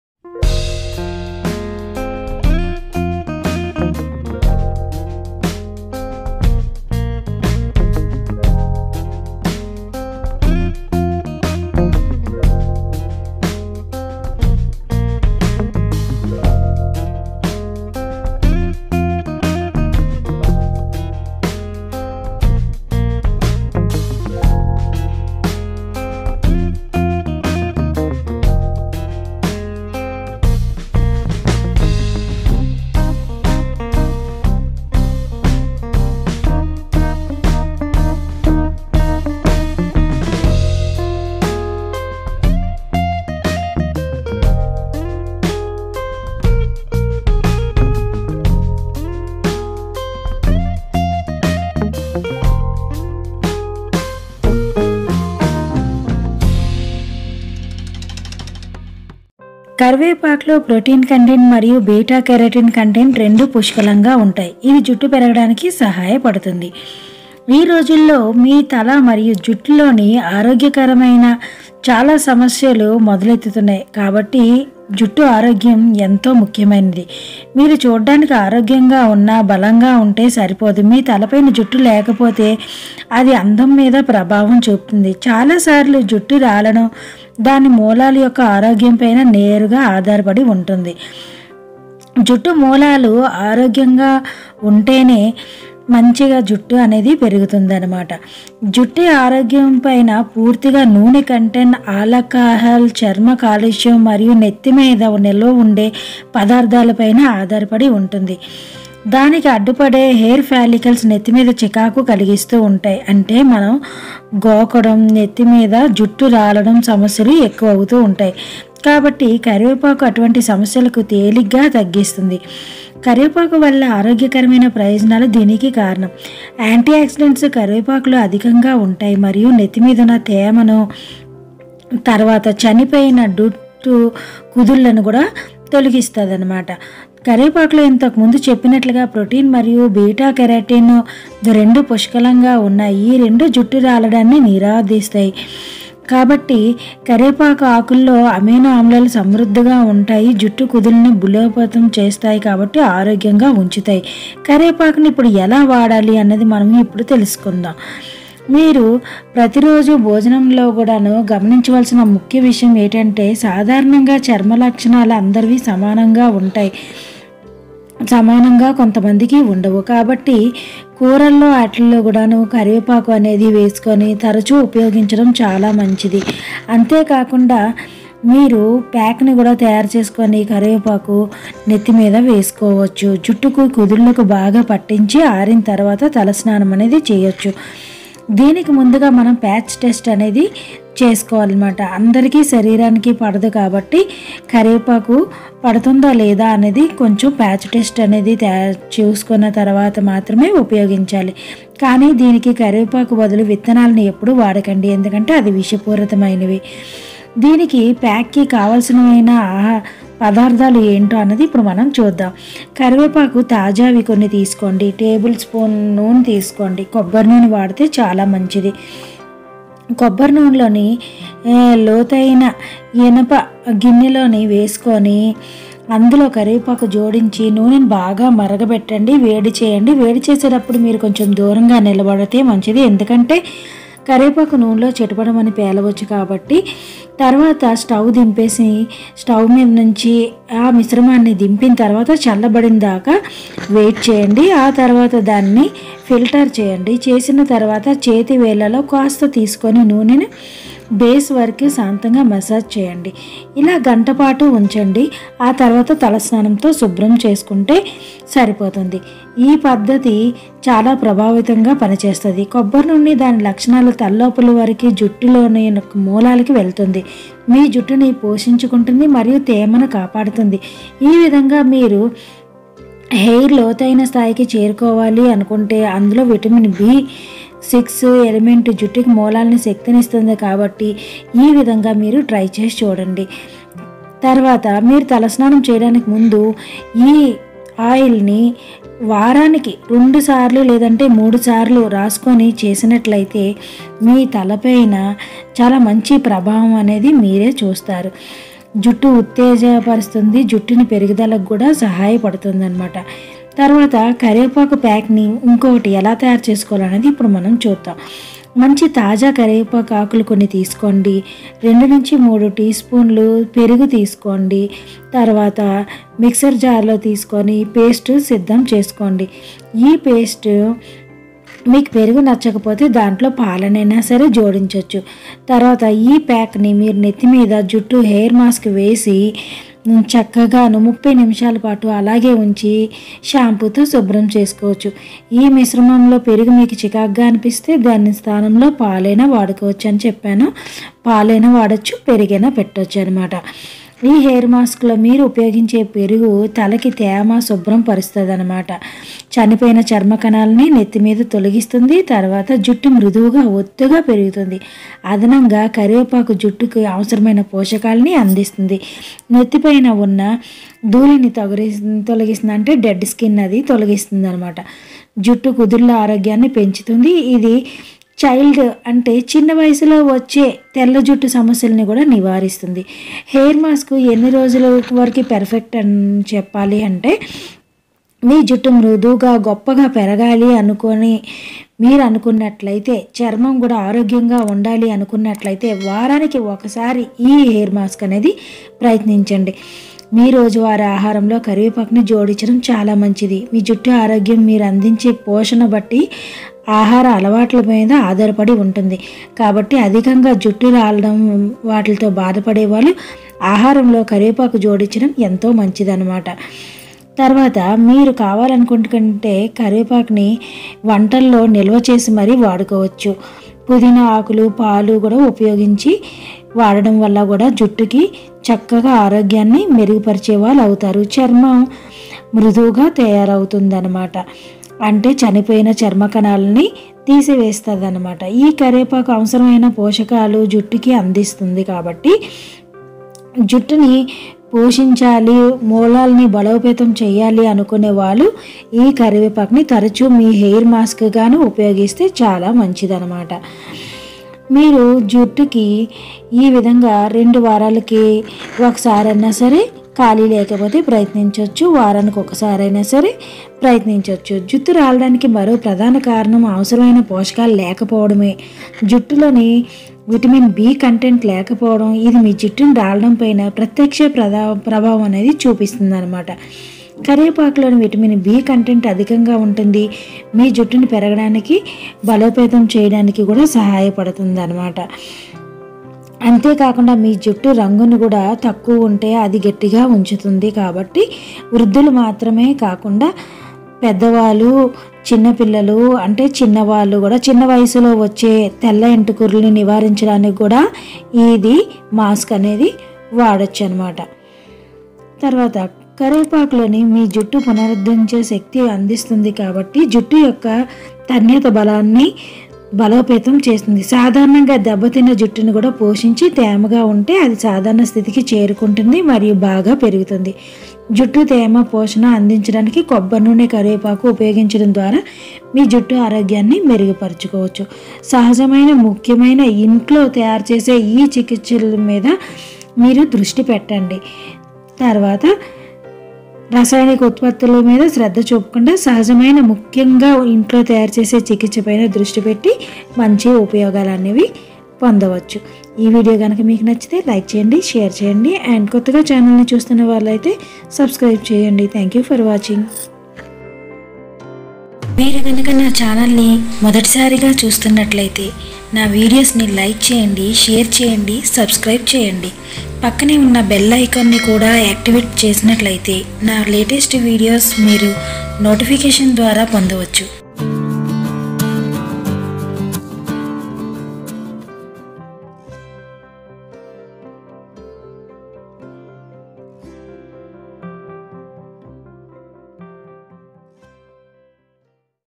Amazing Tips for Hair Regrowth with Curry Leaves In telugu | Curry Leaves For Hair Growth|DIVYASRI
In this video I share u, Amazing Tips for Hair Regrowth with Curry Leaves In telugu | Curry Leaves For Hair Growth|DIVYASRI

For details plz check out this video.